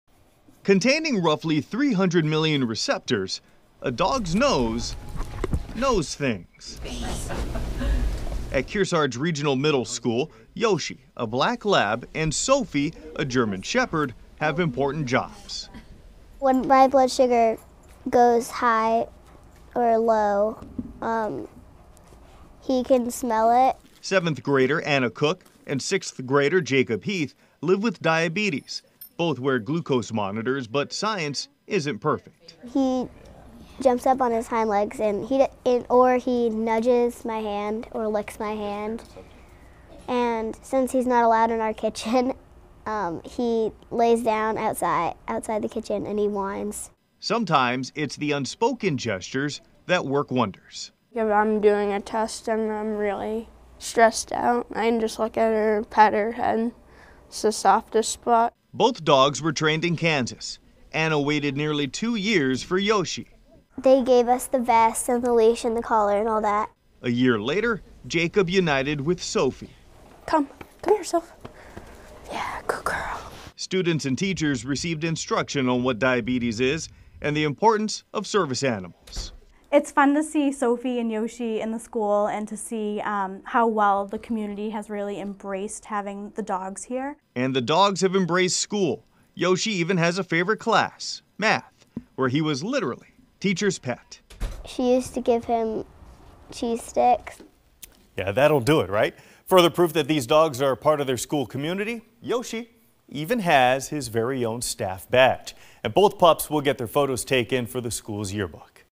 Service dogs teach about differences at school
Two students at Kearsarge Regional Middle School are using their service dogs to teach the school community about the challenges they face.